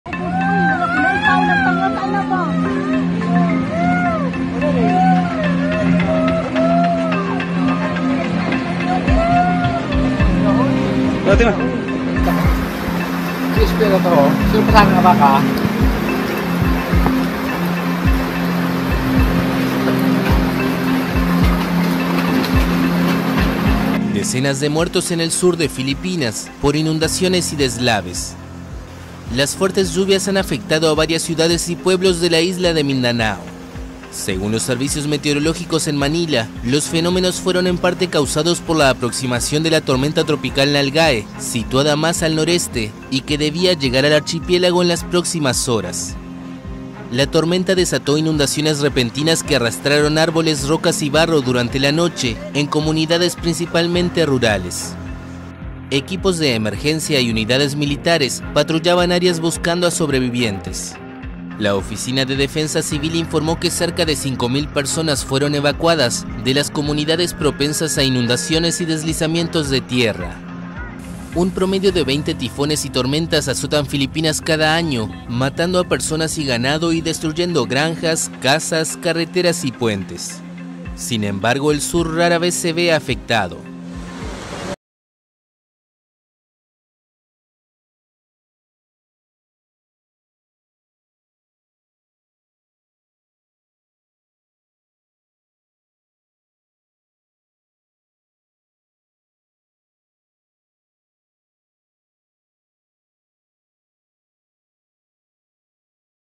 Inundaciones y deslaves mortales en Filipinas ante la llegada de la tormenta Nalgae
Más de una decena de personas fallecieron debido a las inundaciones y deslaves que provocó la llegada de la tormenta tropical Nalgae, en Filipinas. Las intensas lluvias afectaron a varias ciudades y pueblos de la isla Mindanao.